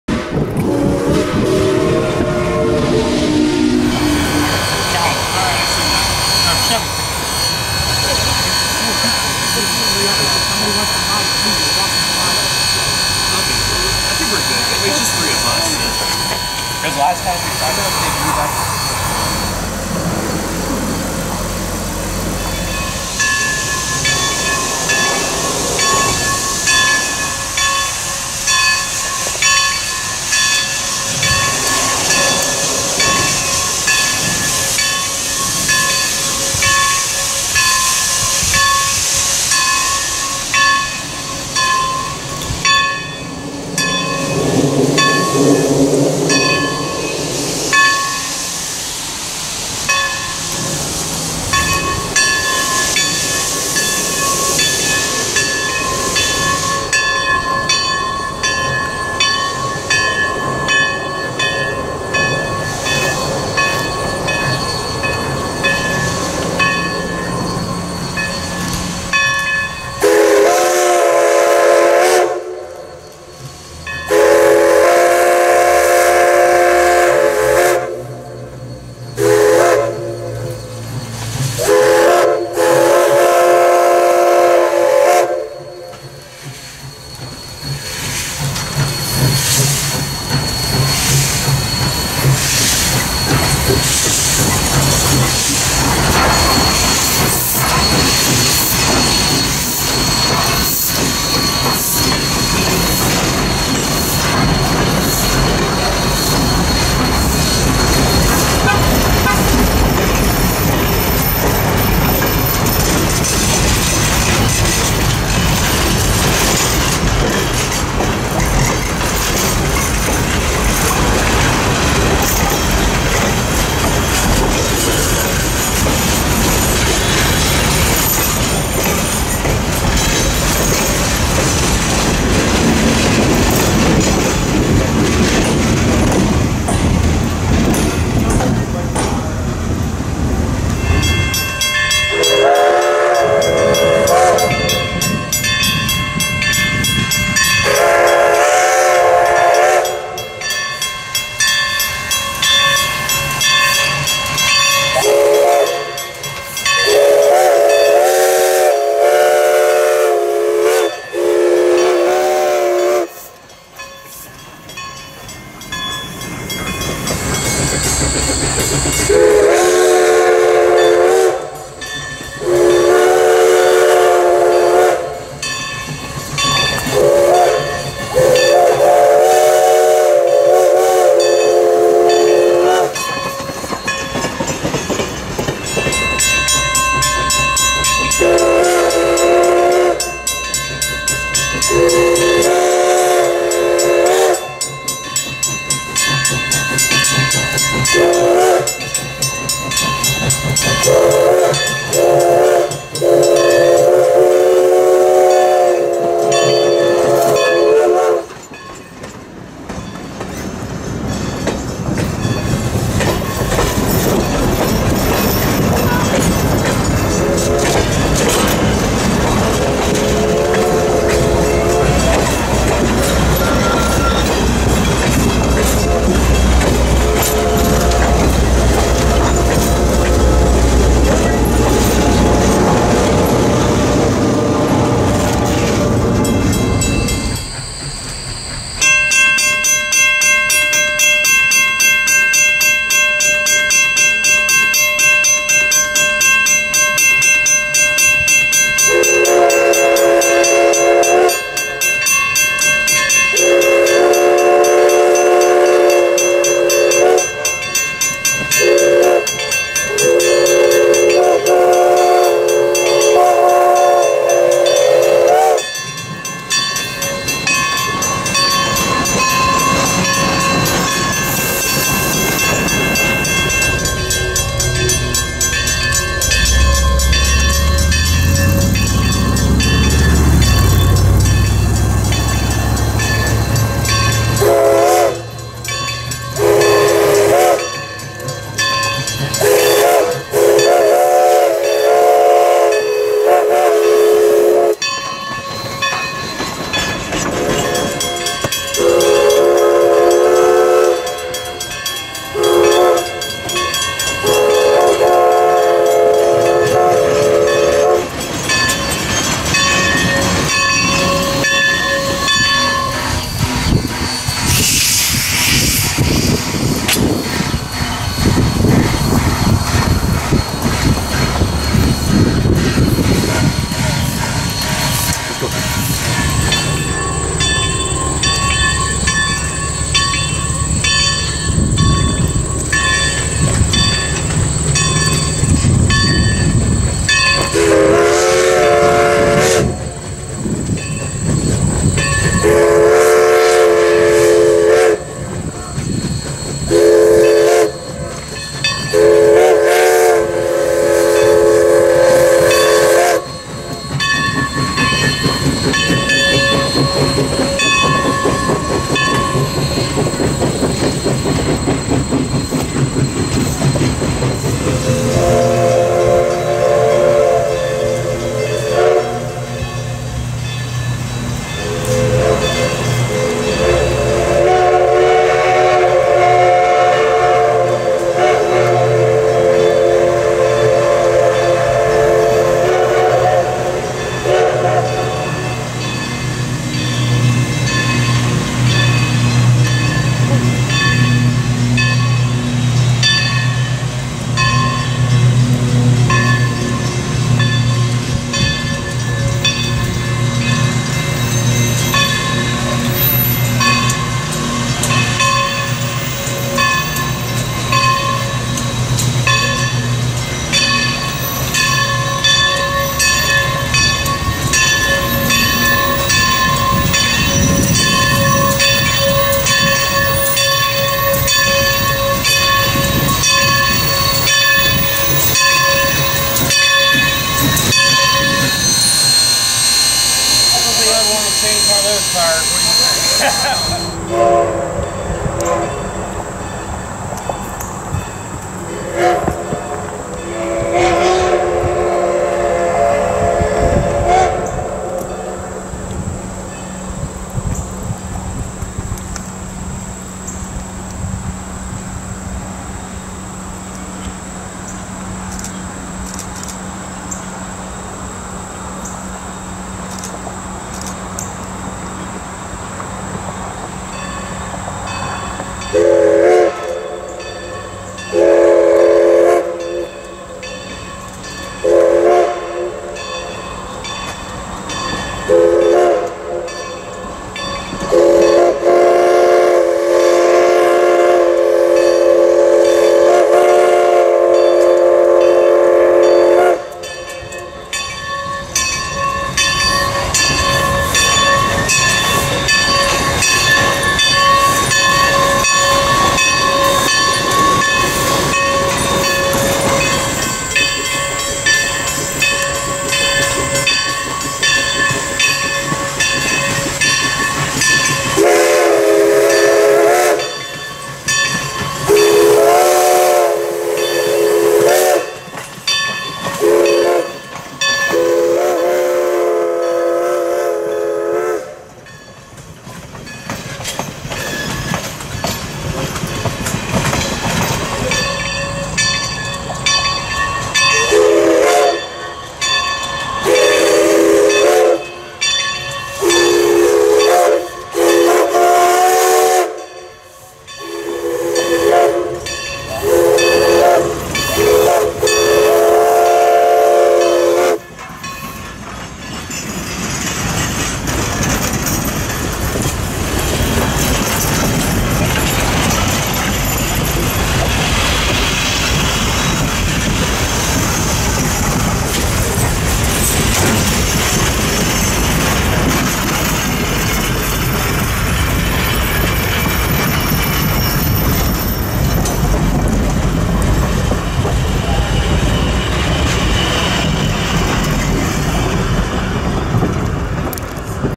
EVERETT RAILROAD STEAM INTO THE COVE 2021
on this episode of steam enthusiast productions we will... be chasing #11 at the everett railroad (a 2-6-0) a mogul from the American locomotive company on it's journey from Duncansville to Martinsburg and to roaring spring